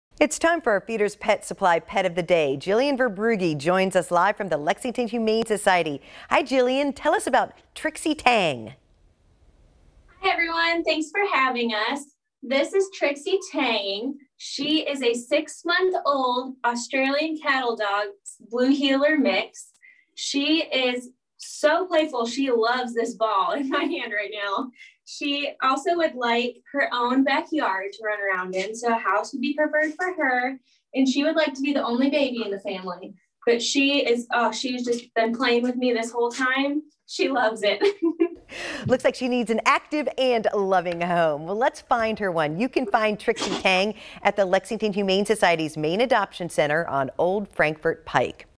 Pet of the Day: Trixie Tang
Our Pet of the Day is Trixie Tang. She is a 6-month-old Australian Cattle Dog/Blue Heeler Mix who does well with children over the age of 10.